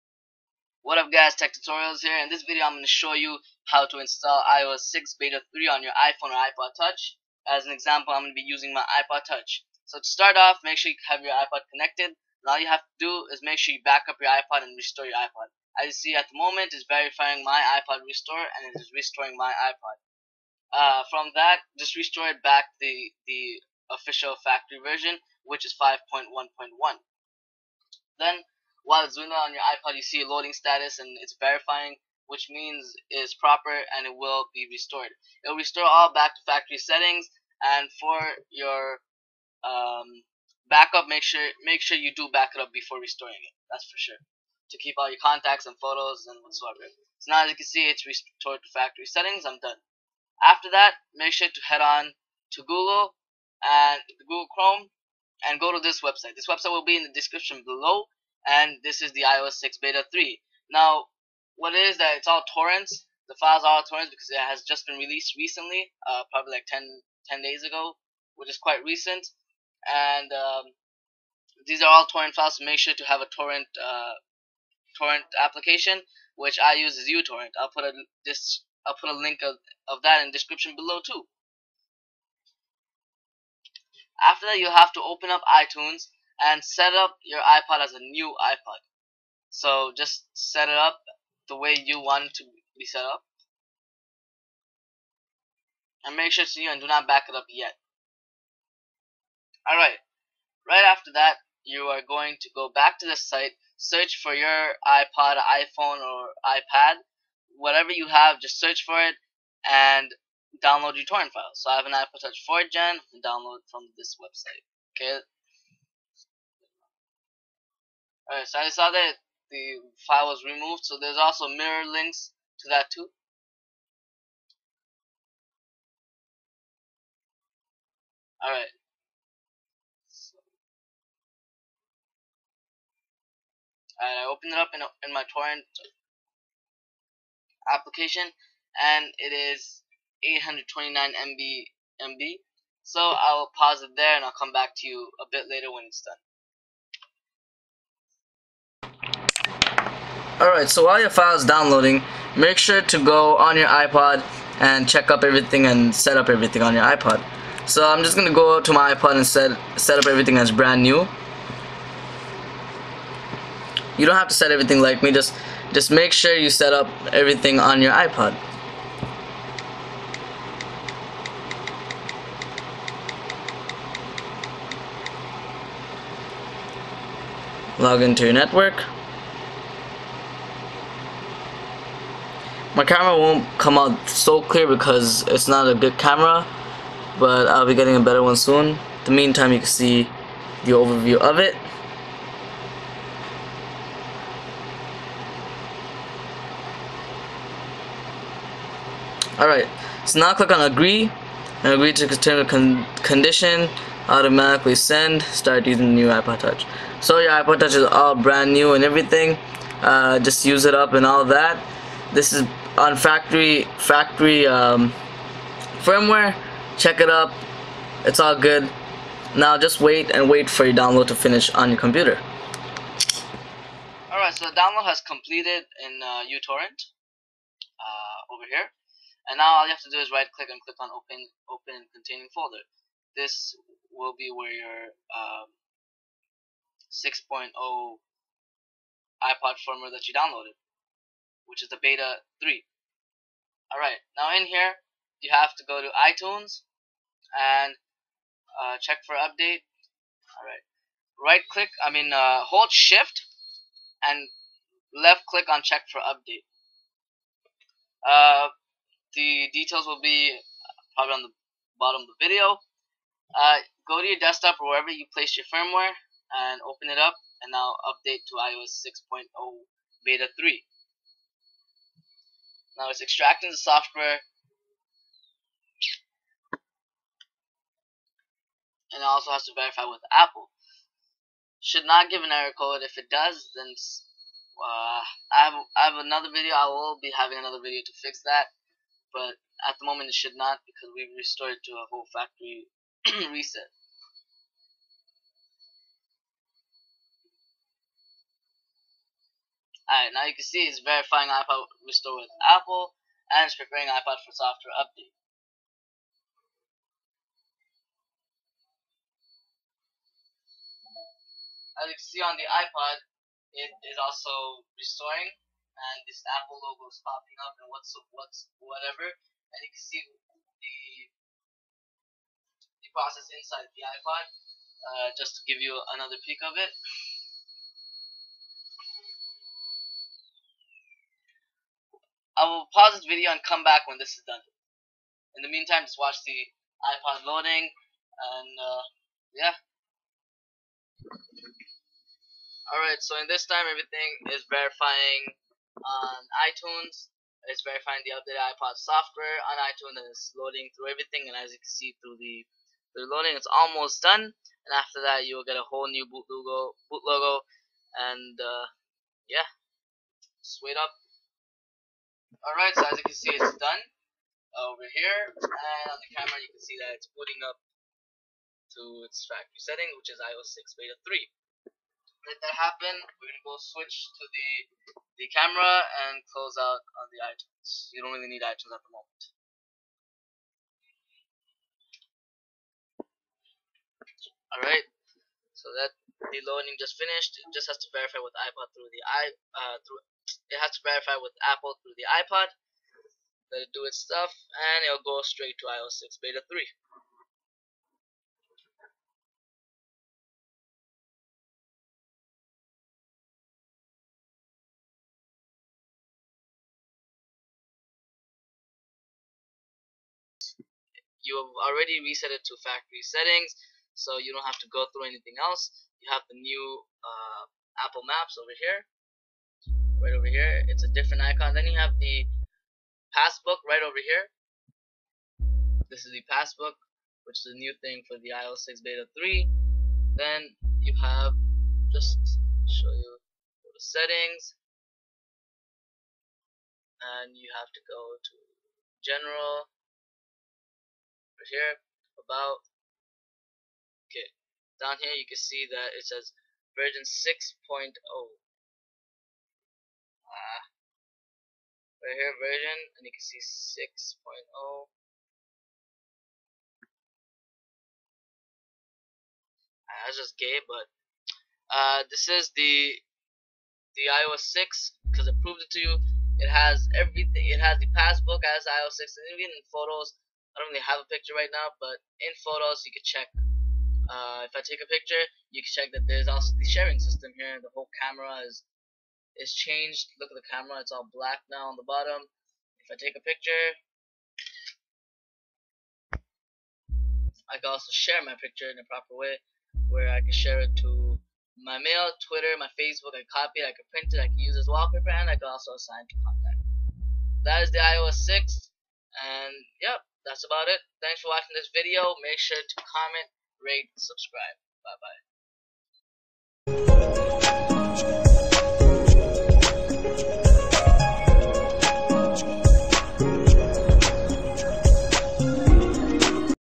How To Install iOS 6 Beta 4/3 Free Without A Developer Account! - [iPhone/iPad/iPod]
If you guys want to become a partner right away!
UPDATE!:
IMPORTANT: If you're already on iOS 6 Beta 3, then head over to Settings - General - Software Update OTA, and you will update it straight to beta 4 from beta 3! :D
Hey Guys, This is a video on how to install iOS 6 beta 3 and 4! on your iPhone/iPod/iPad.
Compatible Devices:
iPhone 4S,
iPhone 4,
iPhone 3Gs,
iOS 6 Beta 3 is the most recent version: "iOS 6 Beta 3 - 7/16/12"
This is to install iOS 6 easily with no Developer Account and you don't need your UDID!
 - At the Moment, there is no jailbreak for iOS 6
Make sure you have the latest iTunes Version (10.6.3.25)
If You Don't know what iOS 6 is, or what will have in its update then click here for an overview of it
TechTutorialsz